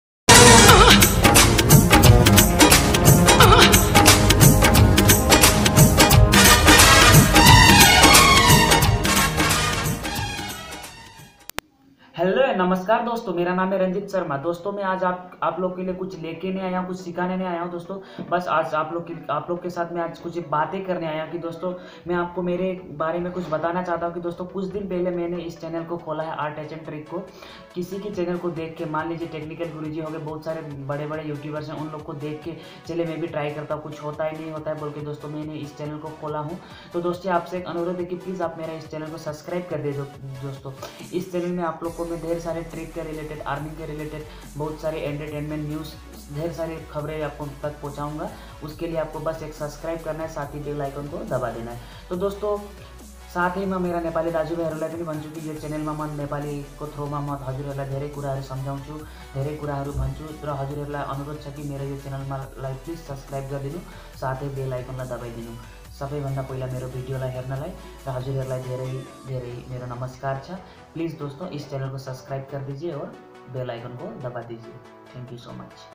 How to promote Youtube channel || nepali channel || nepali technical channel by R Tech tricks
Youtube channel promotion || nepali youtube technical channel
Youtube channal ko promote kaise kare sari jankari apko mil jayegi
  Dosto is video meh maine apni youtube channel ki kuch bate kabi hai is video ko dekhne k liye dhanyawad plzz meri is channel ko subscribe kar dijye taki meh or ache ache video dhal saku

 By R Tech tricks rtechtricks